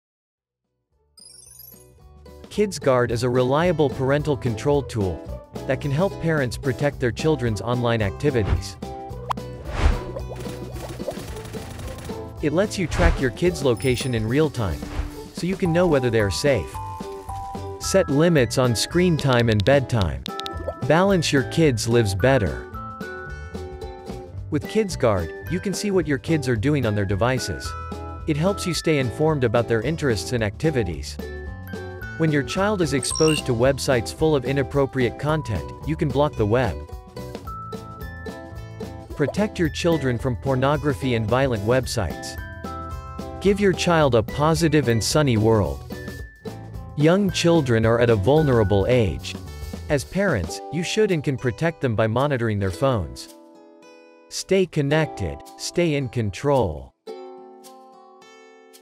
ClevGuard KidsGuard | Wonderful Parental Control Tool
Ensure the safety of your kids' online activities with KidsGuard. ✨It allows parents to track real-time location, and detect inappropriate content. Let's get started!

🧐Got questions or curious to learn more? Jump into our Discord channel for guidance and enjoy exclusive discounts just for our Discord community